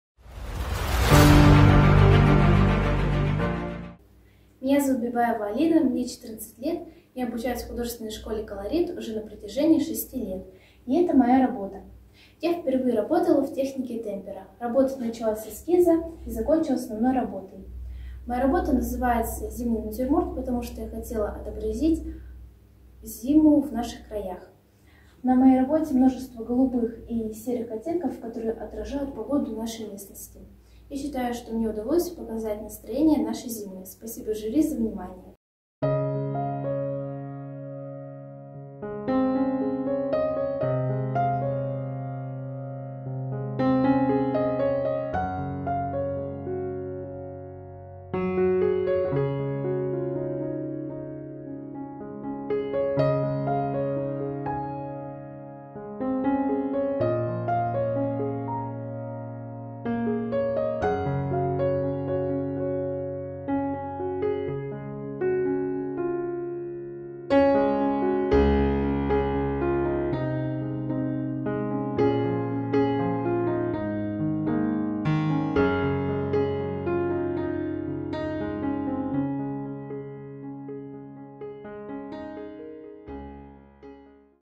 Golden Time Talent | 30 Season | Bibaeva Alina | PAINTING
Имя участника или коллектив: Bibaeva Alina

Страна: Russia

Город: Саяногорск, Центральный микрорайон д. 6кв1

Название конкурсной работы: Зимний натюрморт

Размеры работы: 23х29,5

Материал исполнения: Картон, темпера

Педагог: Миронова Светлана Васильевна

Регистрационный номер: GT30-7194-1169

Номинация: Живопись

30 Season

Участие в других конкурсах и фестивалях / творческие достижения: Всегда принимает участие в школьных и городских конкурсах.